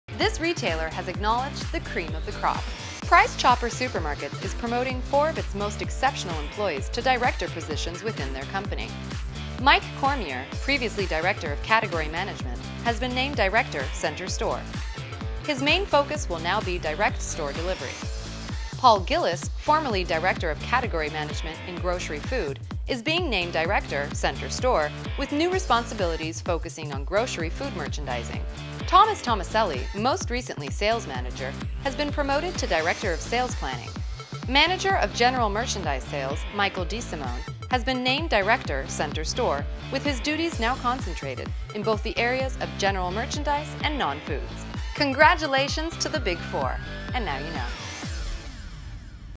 AndNowUKnow: Price Chopper - Four Promotions - News
This retailer has acknowledged the cream of the crop.

Price Chopper Supermarkets is promoting four of its most exceptional employees to director positions within their company.  Mike Cormier, previously director of  category management, has been named director, Center Store. His main focus will now be direct store delivery.  Paul Gillis, formerly director of category management in grocery food, is being named director, Center Store, with new responsibilities focusing on grocery food merchandising.  Thomas Tomaselli, most recently sales manager, has been promoted to director of sales planning.  Manager of  general merchandise sales, Michael DeSimone, has been named director, Center Store, with his duties now concentrated in both the areas of  general merchandise and nonfoods.

Congratulations, to the big four.  And Now You Know.